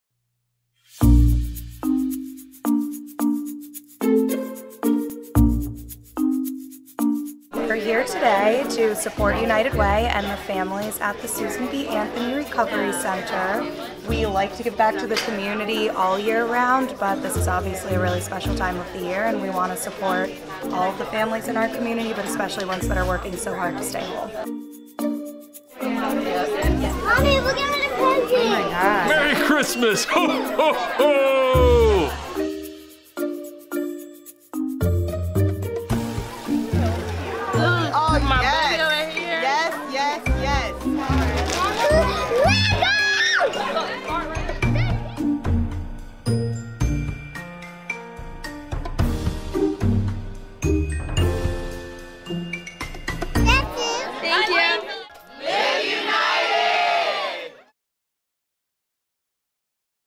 2018 Adopt a Family
Toys, games and gifts of all kinds -- even a bike! -- brought smiles to more than 50 families this holiday season, thanks to the generous donors who participated in United Way of Broward County's Adopt a Family drive. The recipient families are all clients of a United Way partner agency.